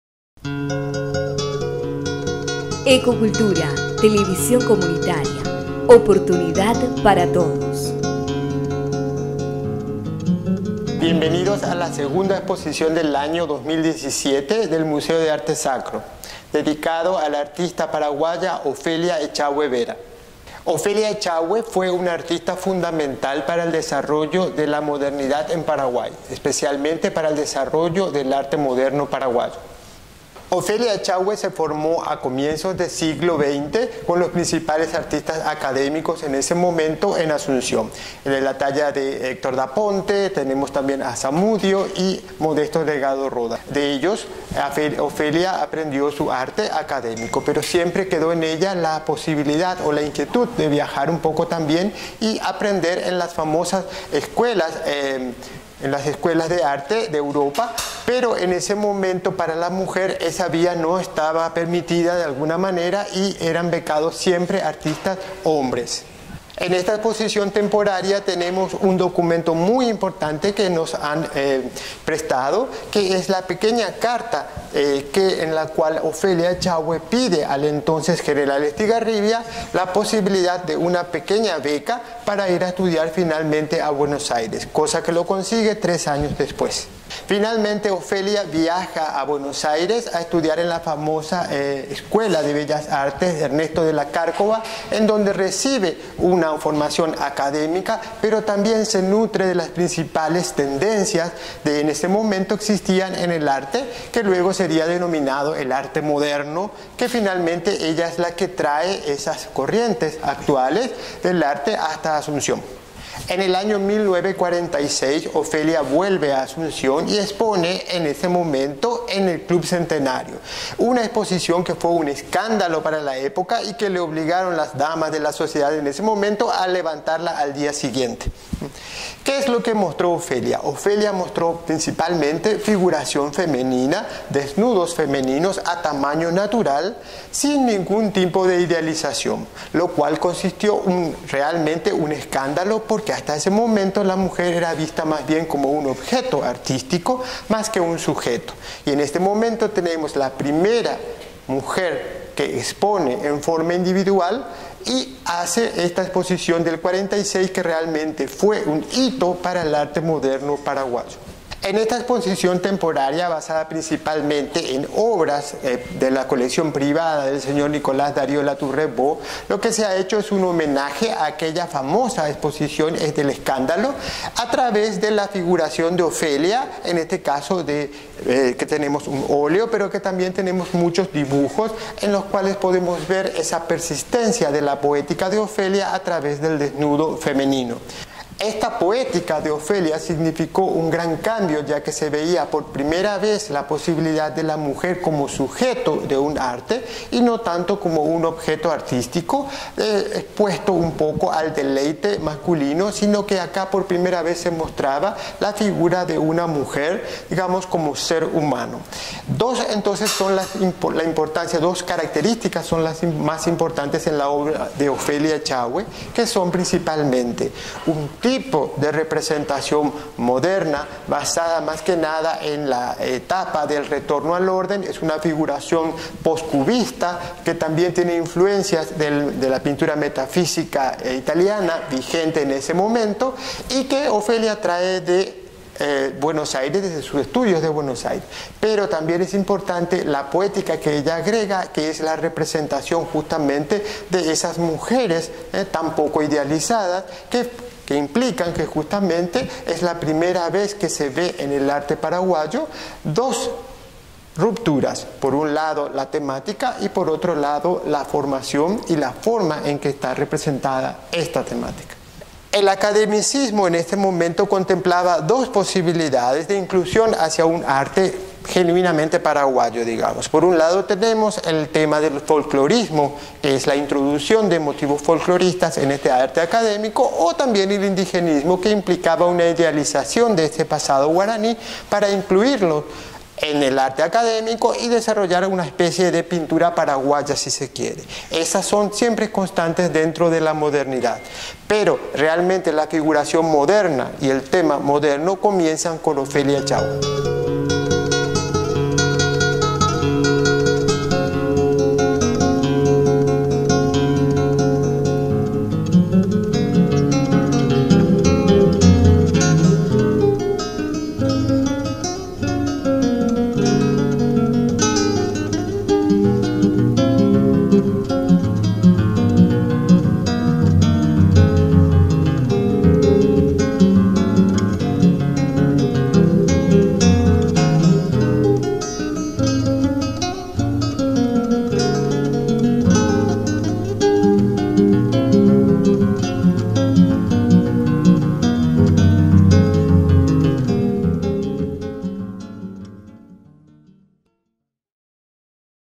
Museo de Arte Sacro, Ofelia Echague y sus pinturas.
una valiosa exposición nuevamente en el Museo de ARte Sacro. Colección del Filántropo Nicolás D. Latourrette Bó. Pinturas de Ofelia Echague, con la notable explicación del Museólogo Luis Lataza. ABierto hasta el  30 de  setiembre.